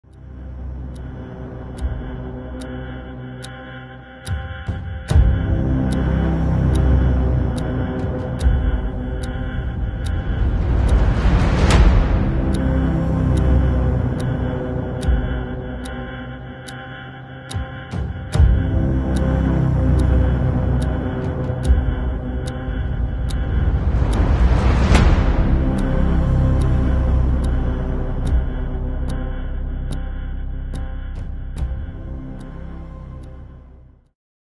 Прикордонна пошта доставляє вибухові посилки окупантам на другу лінію
У результаті отримані замовлення знищили два ворожих укриття, авто та склад боєкомплекту загарбників на Півдні.